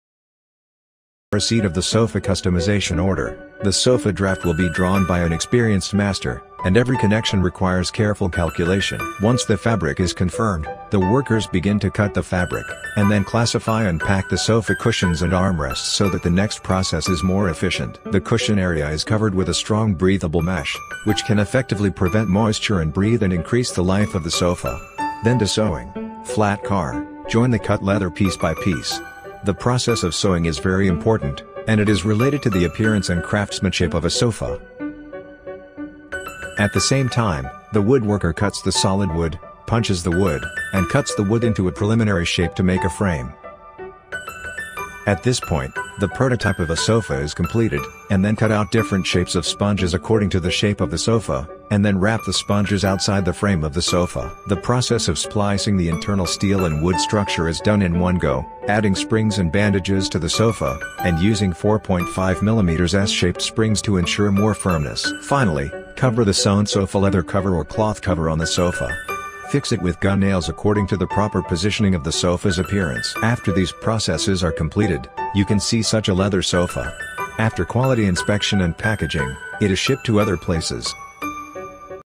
The Birth Of A High-Quality Home Theater Sofa: Made By Chihu
To put it simply, it is first to design the sofa theme drawing, select materials, and then assemble. First, sew the cloth, make the internal steel-wood structure, then install the spring, bandage, and cotton on the steel-wood structure, and finally put the cloth on the steel-wood structure to complete the whole sofa production process.